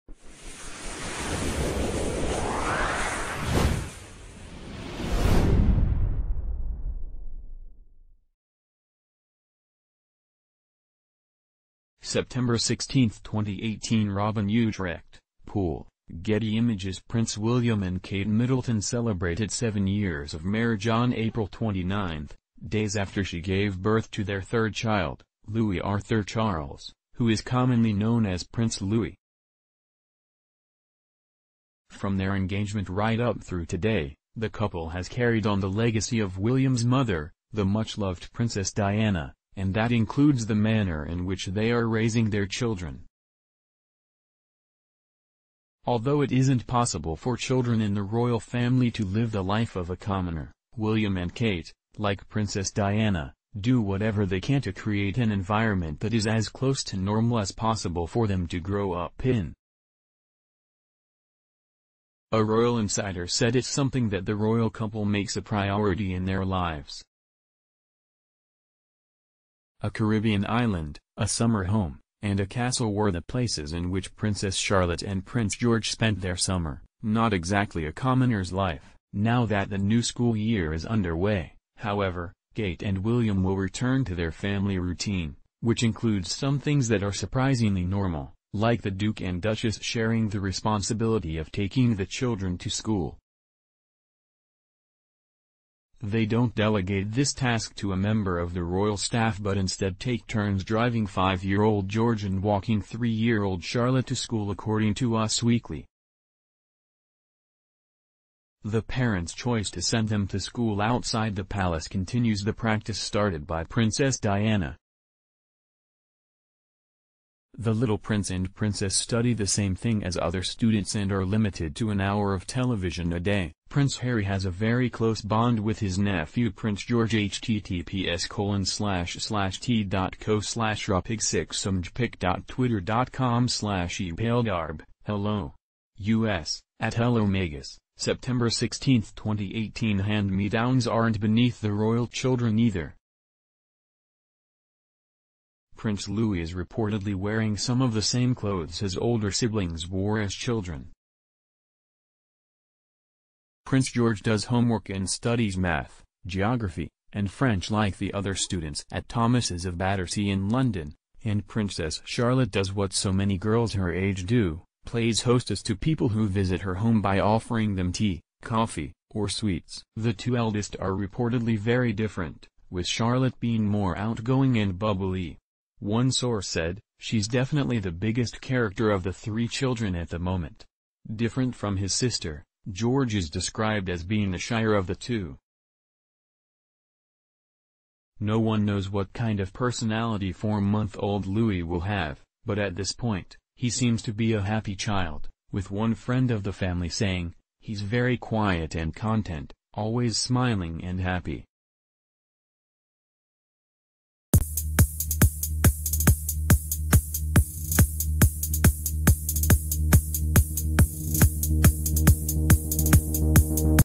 Prince William & Kate Middleton Raise Their Children In An Environment That’s As Close To Normal ...
Prince William & Kate Middleton Raise Their Children In An Environment That’s As Close To Normal As Possible
September 16, 2018 Robin Utrecht - Pool / Getty Images Prince William and Kate Middleton celebrated ...